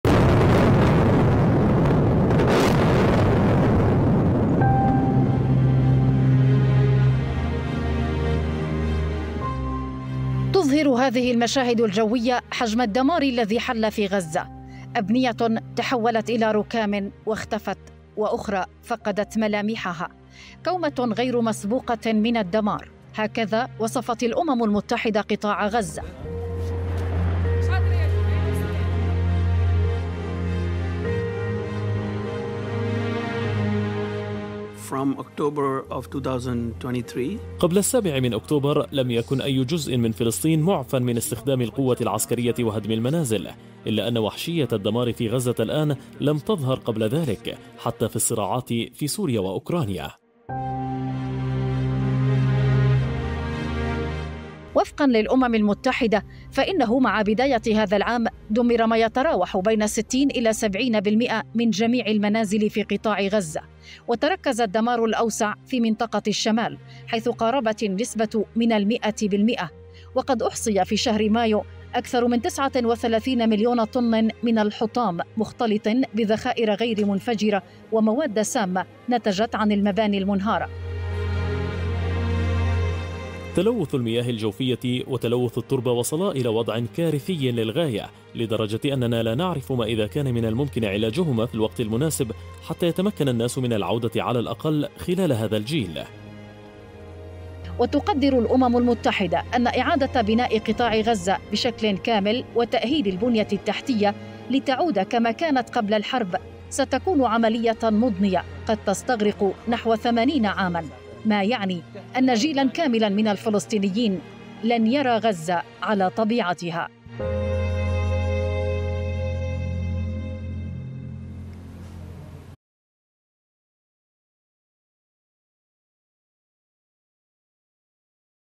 الأمم المتحدة: تدمير 70% من منازل غزة.. وإعادة إعمار القطاع تتطلب 80 عاما
كومة غير مسبوقة من الدمار.. الأمم المتحدة: تدمير 70% من منازل غزة، وإعادة إعمار القطاع تتطلب 80 عاما

لمعرفة أحدث المستجدات.. عبر البث المباشر من هنا
وبإمكانكم متابعة الحدث على منصات التواصل